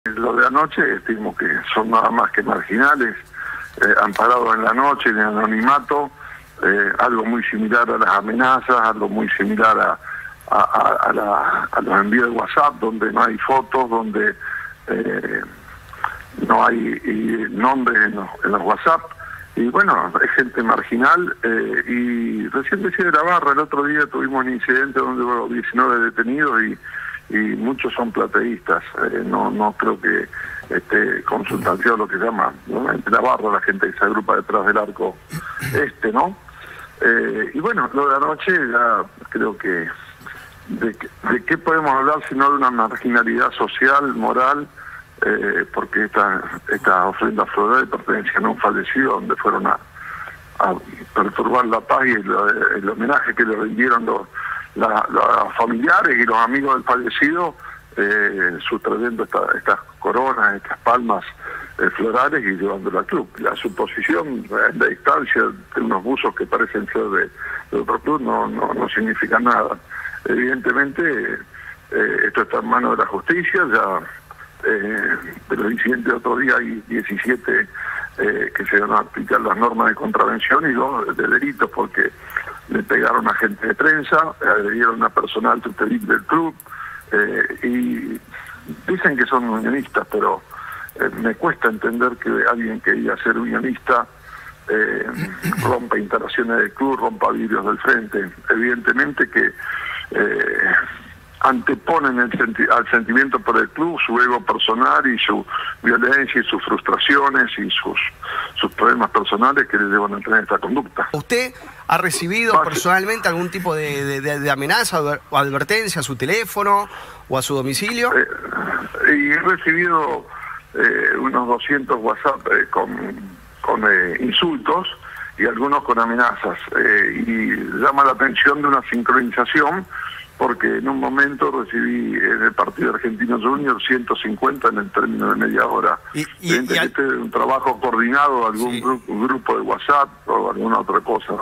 APARECIERON CORONAS FLORALES EN LA PUERTA DEL CLUB UNIÓN
LUIS SPHAN- PRESIDENTE UNIÓN